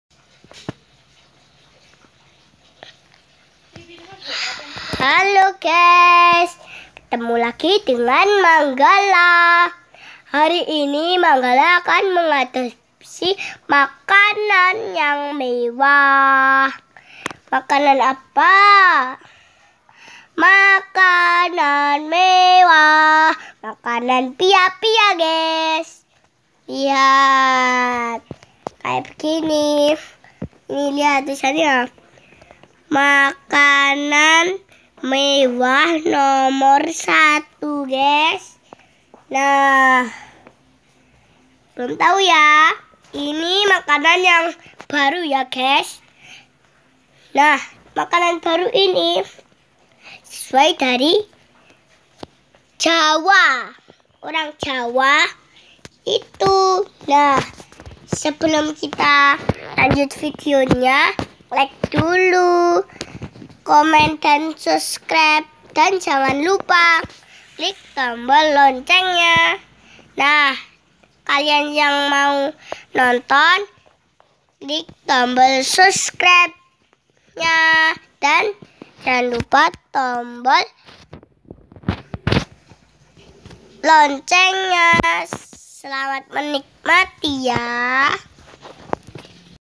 Belajar ngevlog
Manggalla anak berusia 7 tahun belajar ngevlog menggunakan hp papanya